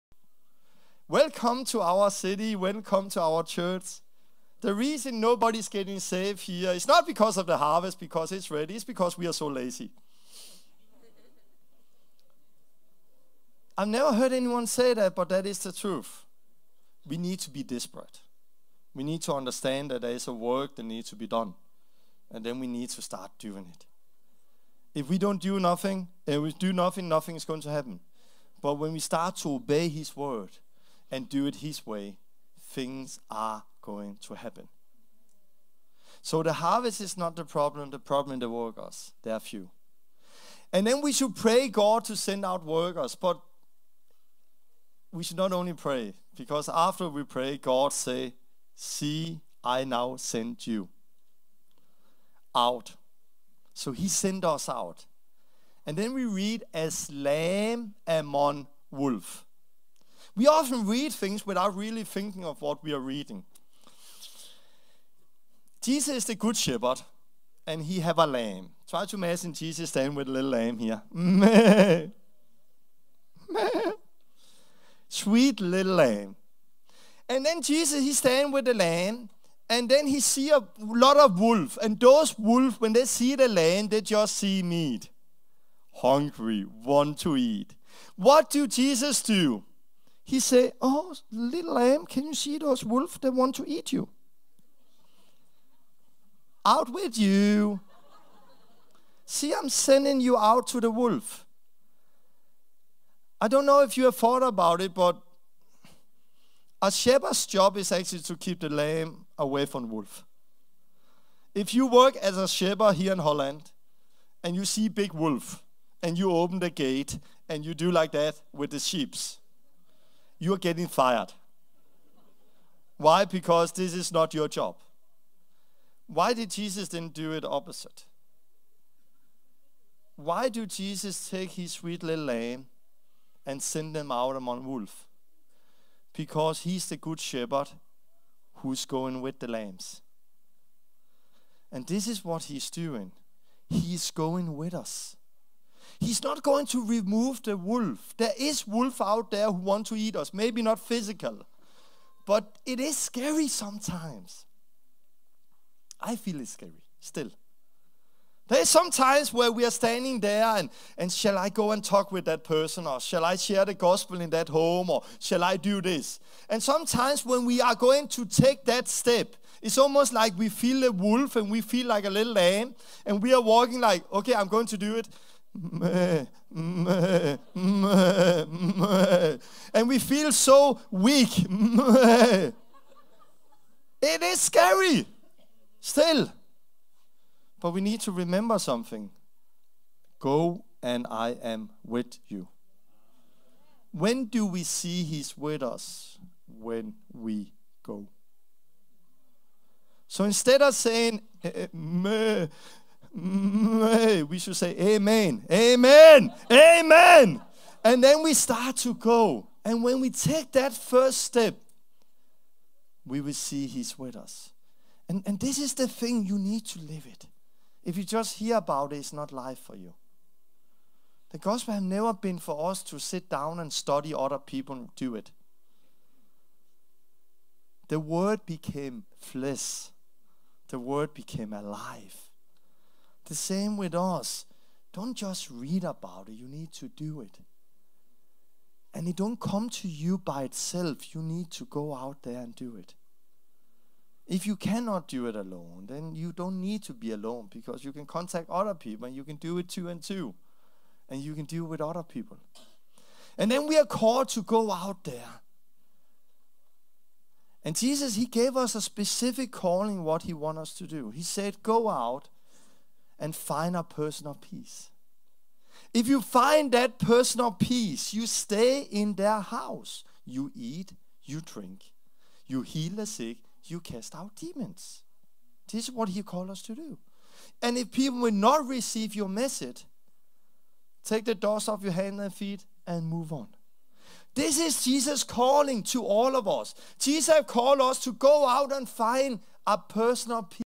2018 Gehet hin; siehe, ich sende euch als die Lämmer mitten unter die Wölfe. Lukas 10,3
Ausschnitt aus dem Original: The Last Reformation: Christians vs. Disciples - Kickstart seminar in The Netherlands, June 2018